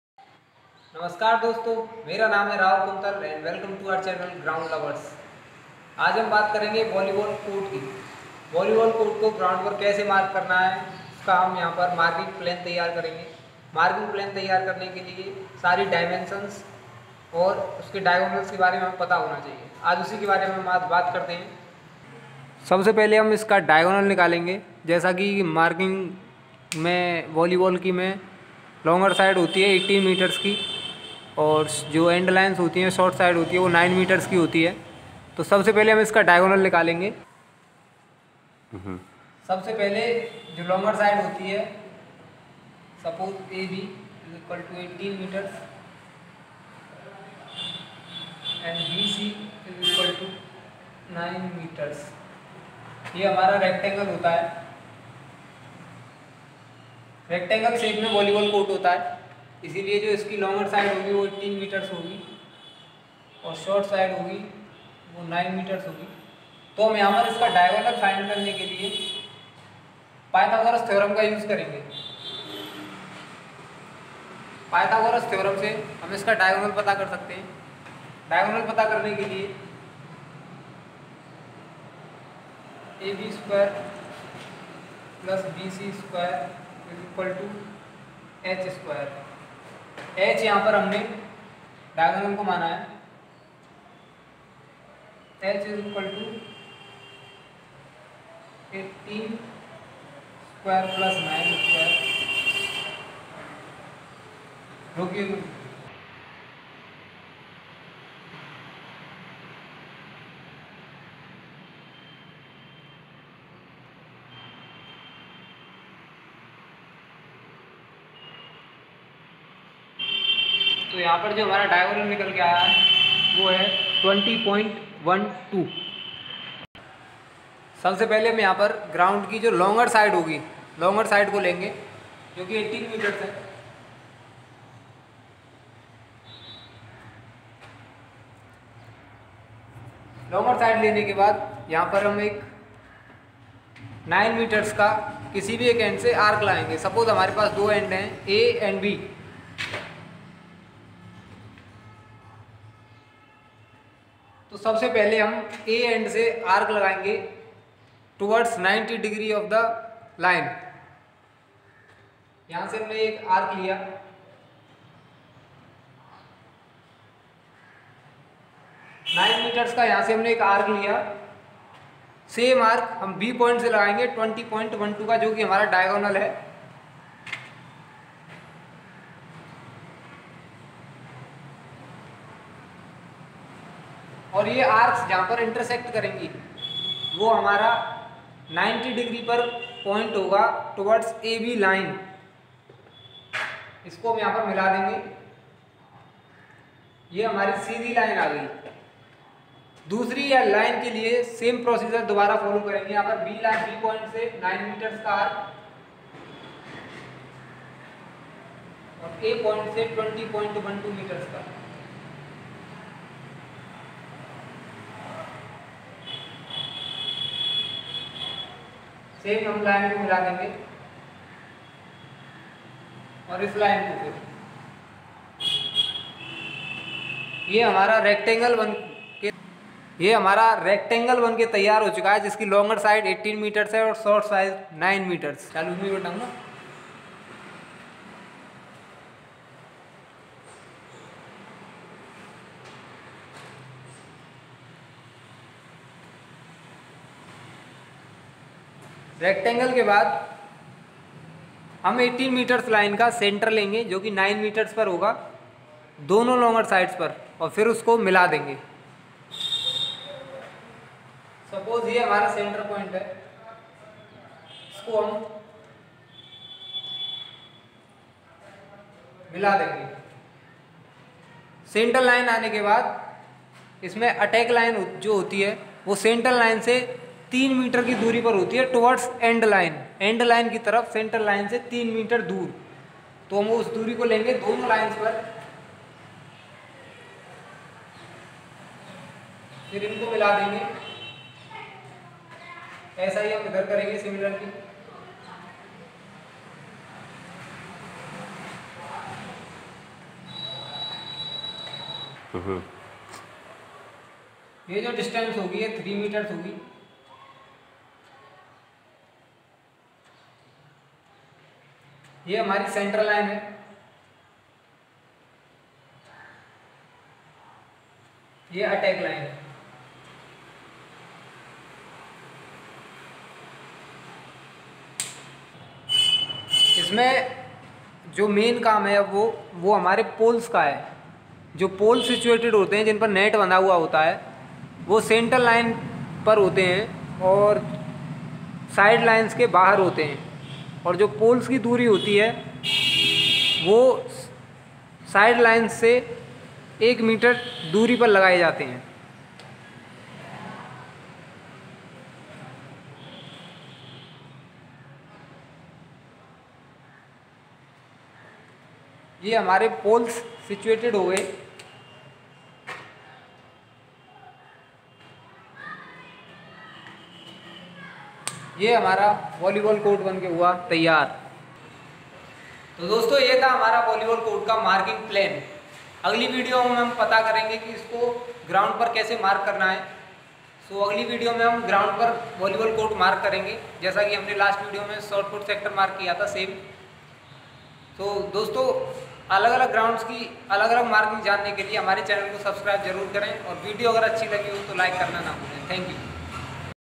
Volleyball court marking plan and volleyball court making
How to mark volleyball court and what are the dimensions of volleyball court..